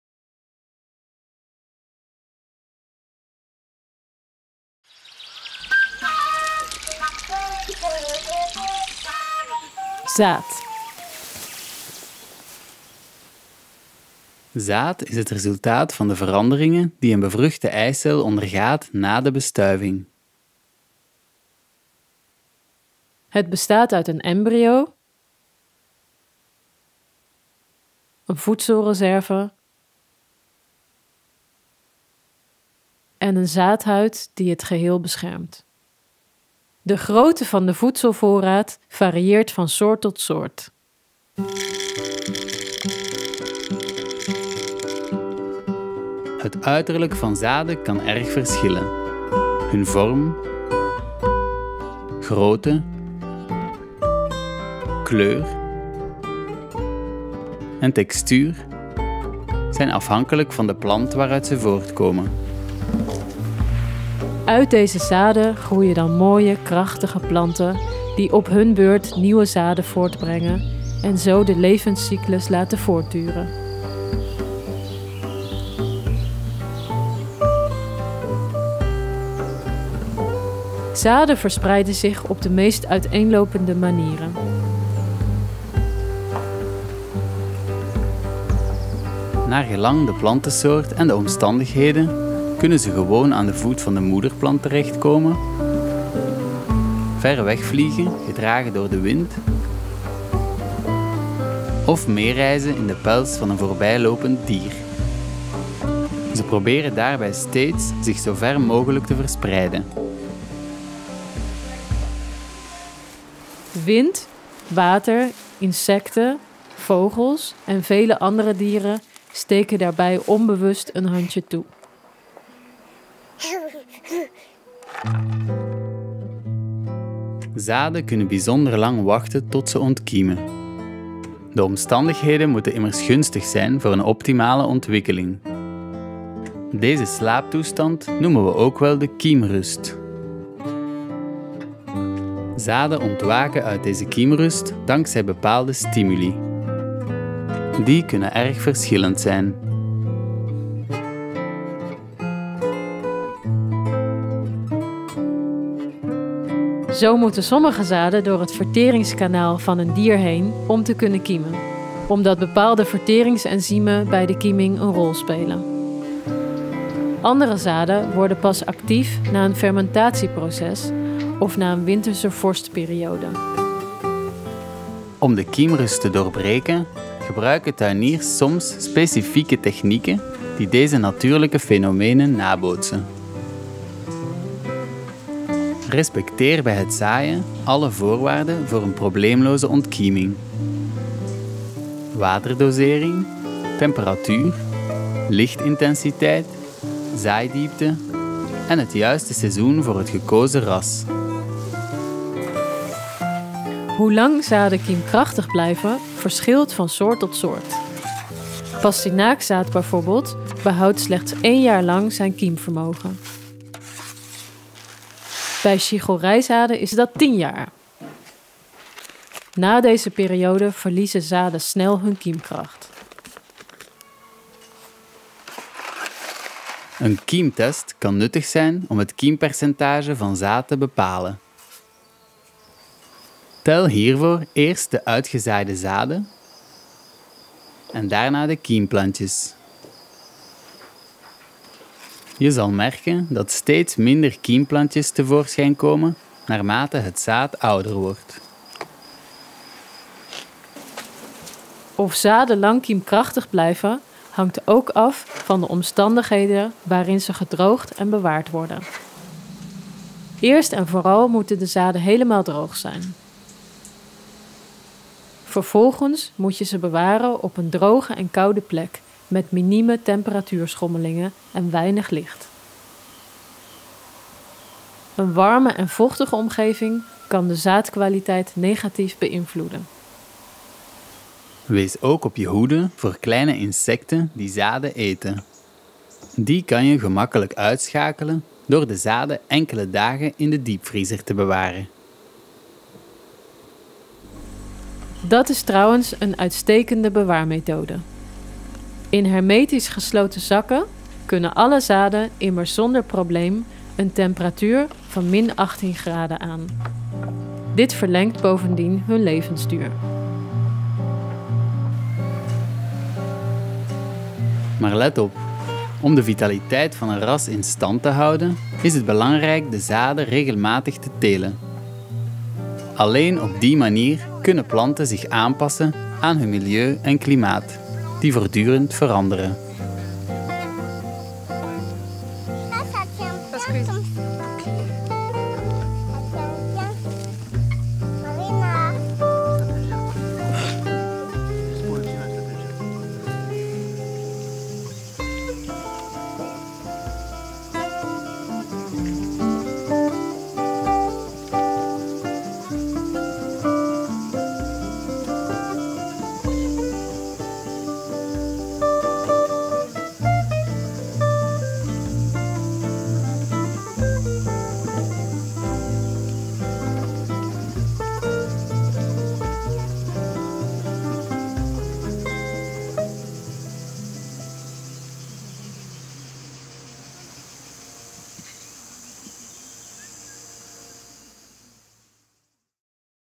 ABC Wat is zaad?
- In dit filmpje leer je wat zaad is: welke delen we onderscheiden en hoe zaden zich verspreiden, ontkiemen, zich ontwikkelen en voortplanten.

Deze video maakt deel uit van ZAAIGOED in OVERVLOED, een videohandleiding om zelf zaad te telen - een reeks van 40 filmpjes die voor meer dan 40 groenten alles uitleggen wat je moet weten om zelf vruchtbaar zaad te kweken.

Alle video's, meer informatie, volledige tekst en vele andere talen op